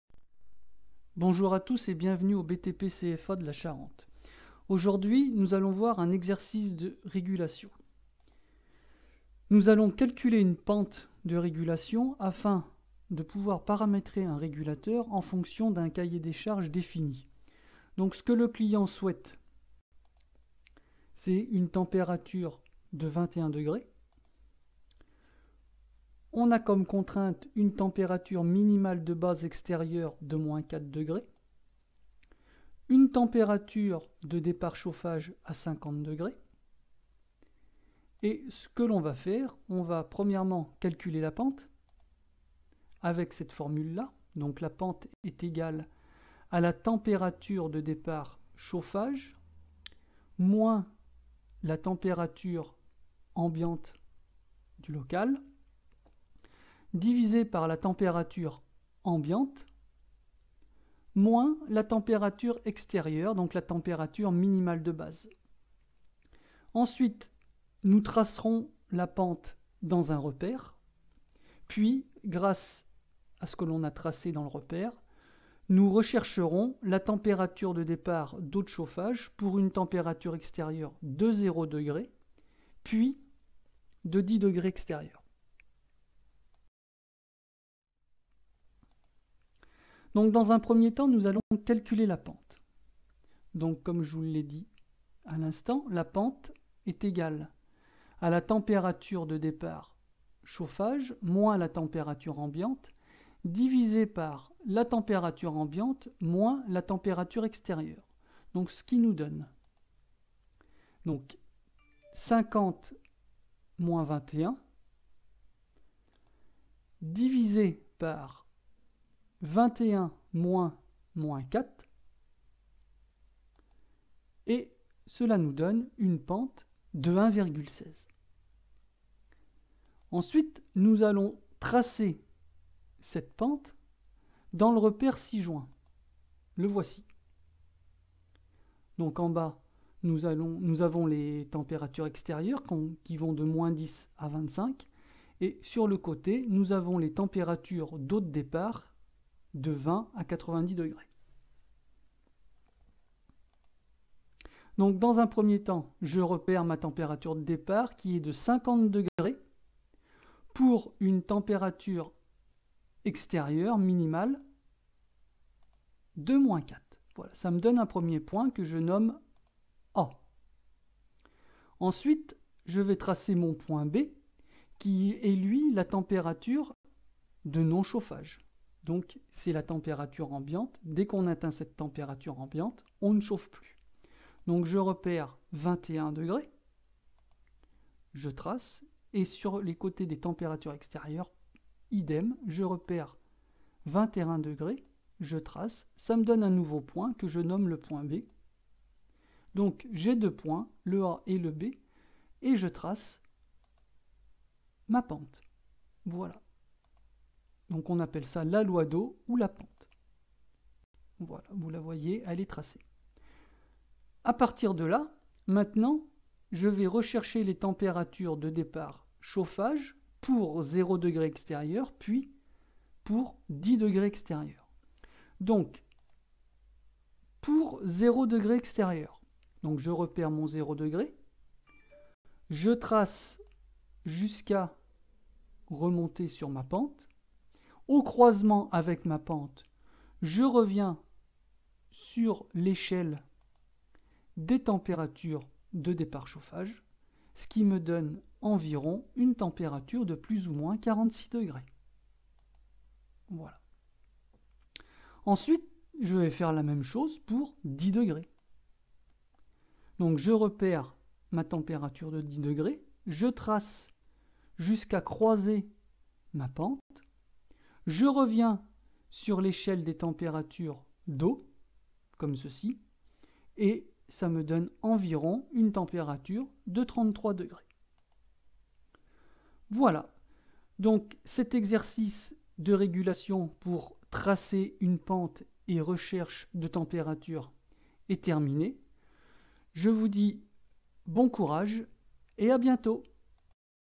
Exercice: Loi d'eau, Pente, Courbe de chauffe
Voici un petit exercice sur la loi d'eau aussi appelée : Pente ou courbe de chauffe.